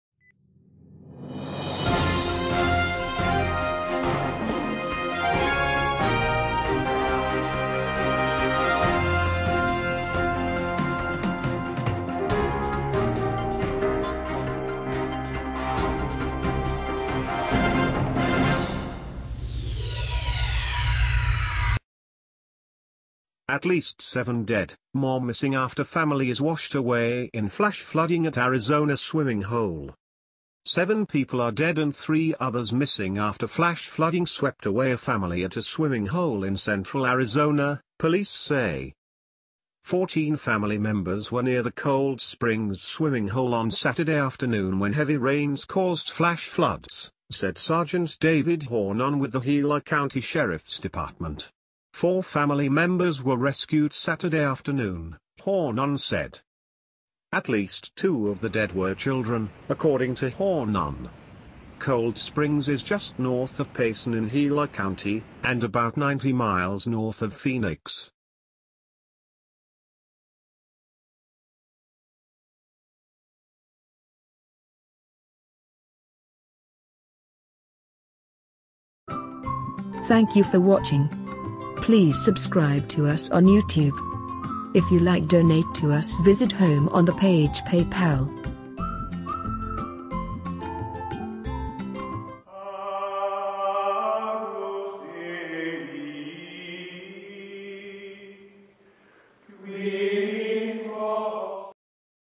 Breaking Arizona swimming hole flash flood
Flash Flooding at Arizona Swimming Hole.
after flash flooding swept away a family at a swimming hole in central Arizona, police say.  the Cold Springs swimming hole on Saturday afternoon when heavy rains caused flash floods, said Sgt. David Hornung with the .

Cold Springs is just north of Payson in Gila County, and about 90 miles north of Phoenix.